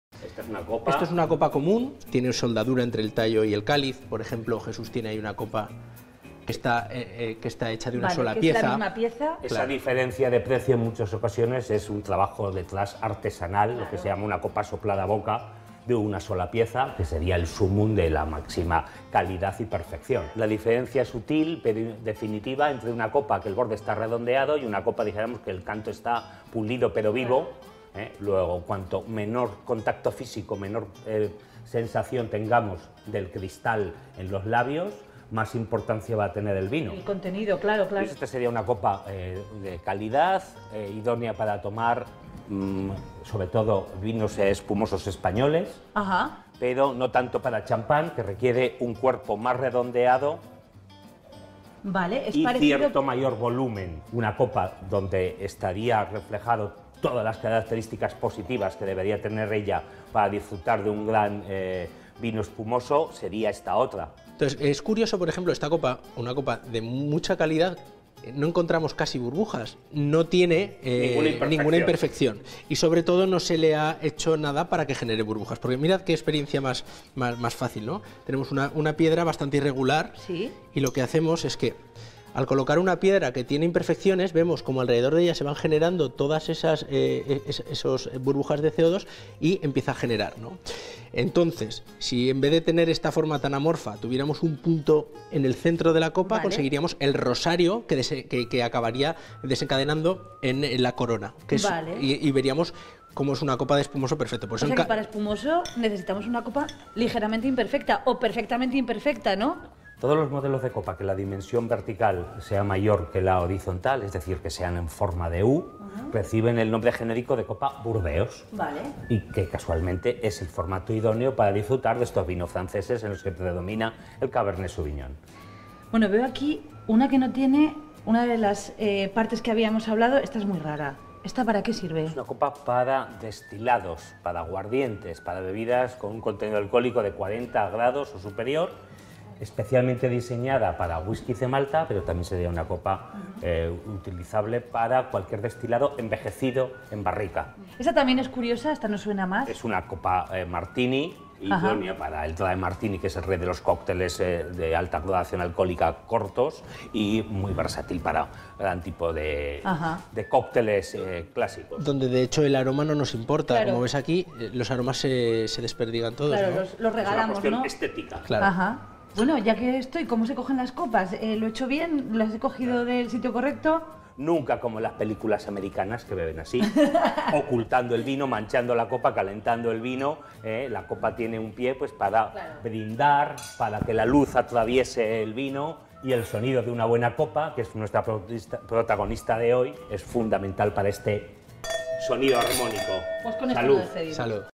¿Sabrá María de Miguel diferenciar los tipos de copa ?
Las copas pueden tener muy diversas formas según su “fuste” y su “cáliz” y ¡Bien Dicho! aprende a distinguirlas bien en Zaragoza. Desde una copa “Riesling” a una “Burdeos”, todas tienen sus peculiaridades, ¿sabrá María de Miguel diferenciar los tipos de copa ?